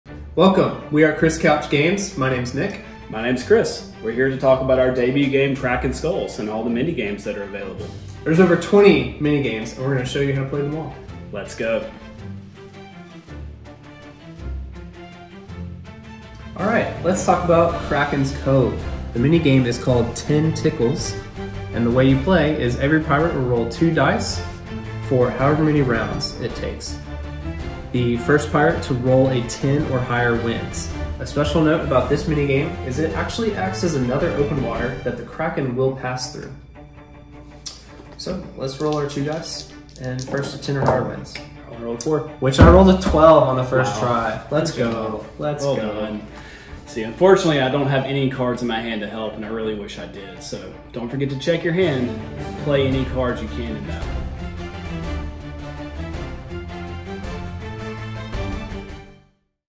Kraken Skulls: Kraken's Kove
This video covers the Kraken Skulls minigame "Kraken's Kove".

Learn all about Kraken Skulls with this playlist of short videos. Use it to learn, teach, and as you play when you need some quick input on what to do next.